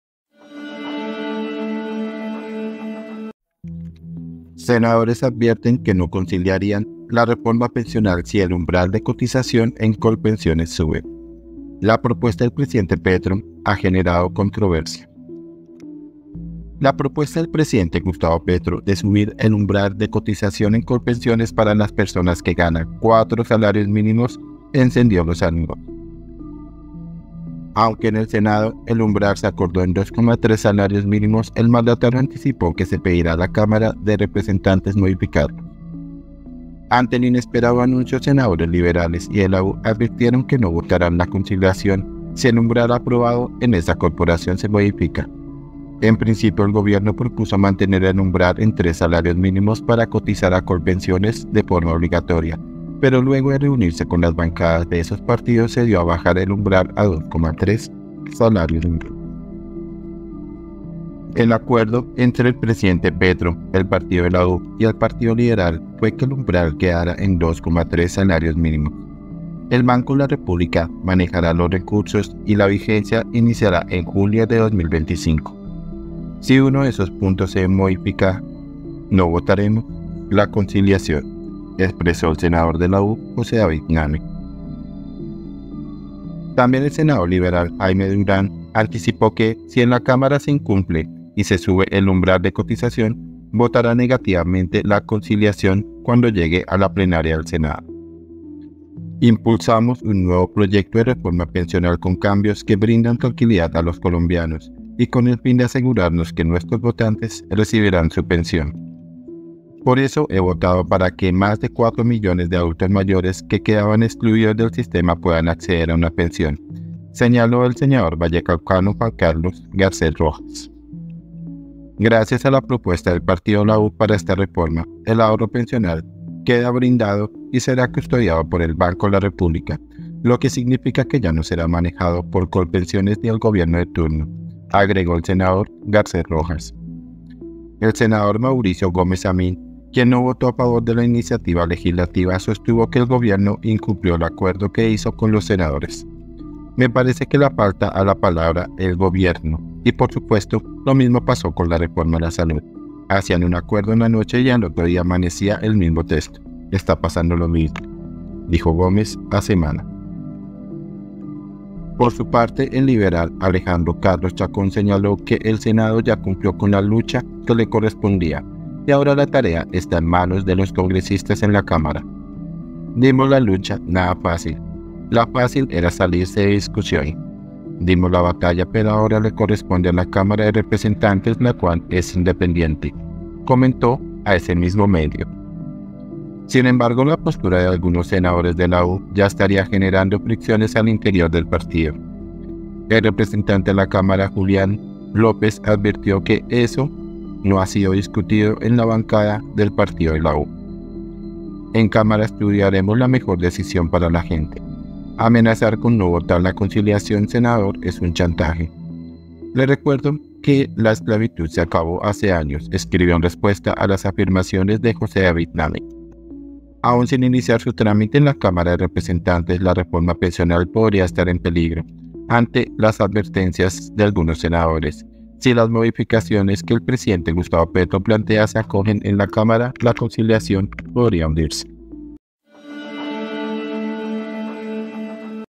REFORMA PENSIONAL EN PELIGRO ⚠️ Senadores advierten que no conciliarían si el umbral sube
Senadores advierten que no conciliarían la reforma pensional si el umbral de cotización en Colpensiones sube
La propuesta del presidente Petro ha generado controversia.